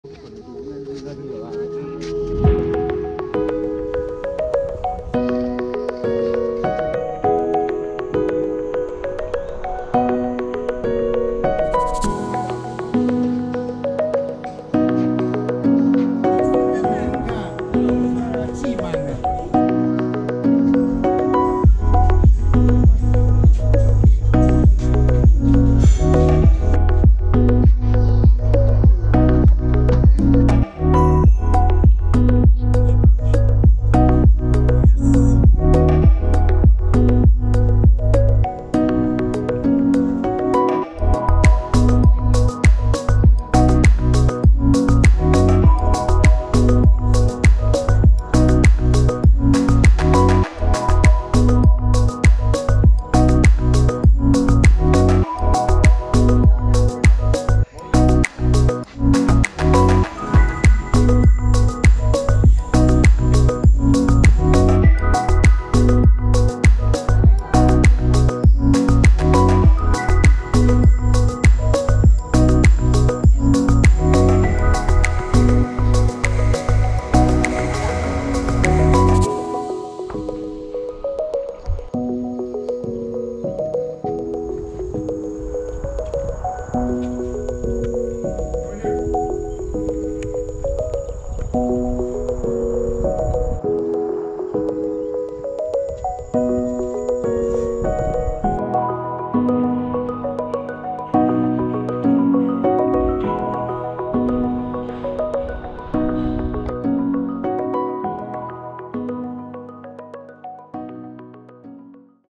The Amazing Tomb Raider Temple | Ta Prohm, Angkor Wat
Ta Prohm, in Siem Reap Cambodia, inside of Angkor Wat, might be the most amazing standing temple in the world. It's easy to get lost in the temple while exploring it for hours.

Video Taken With:
GoPro Hero 6:

-3 way arm ($15):